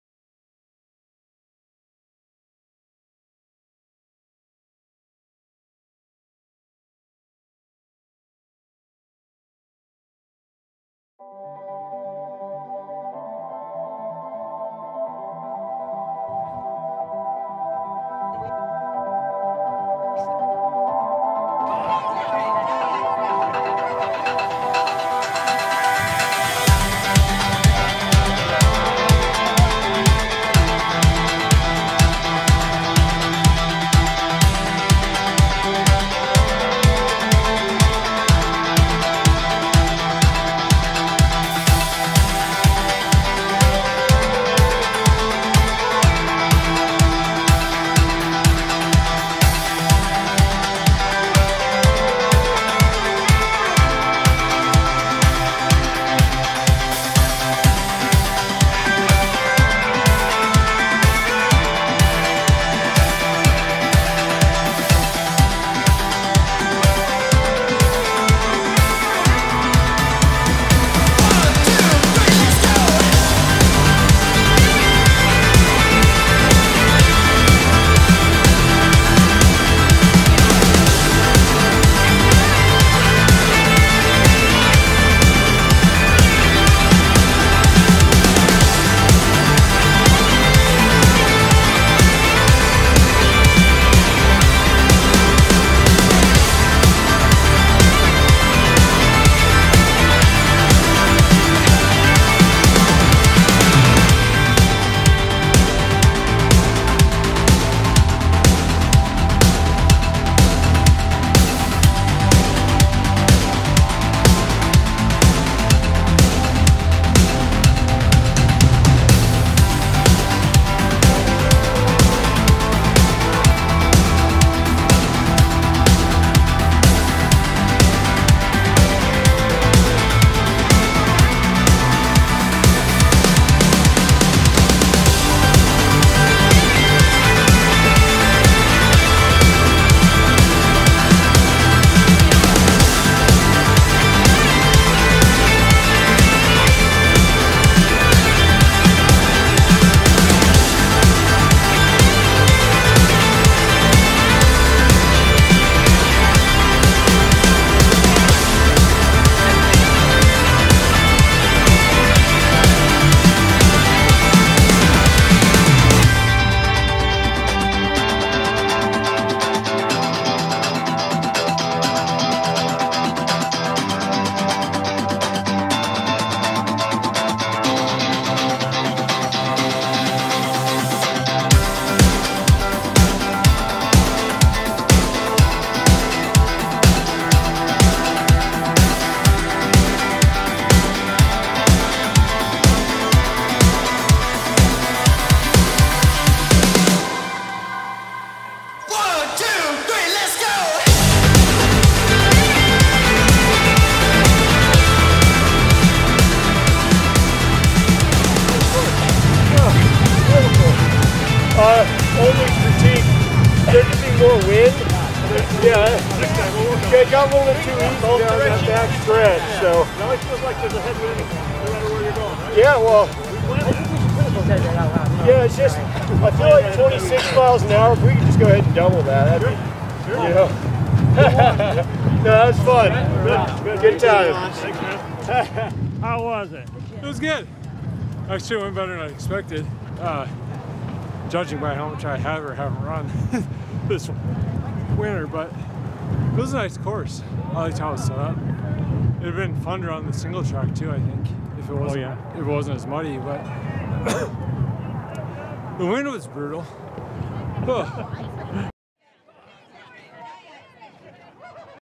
Iowa Trail Running "Red Shamrock Trail Challenge 23" MidWest Trails, No Coast Trail Series
New changes in 2023 for the 12th Annual Team Shamrock Trail Challenge.  They added a 10 mile distance and moved the race to a new place, Coralville Creekside Park! The 5K and 10 mile will have the same start/finish line.
A great way  to start the No Coast Trail Series!

It was a very very windy day!!!

Good Luck and Have fun this  running season!!

KYXOOSJWICUURFHK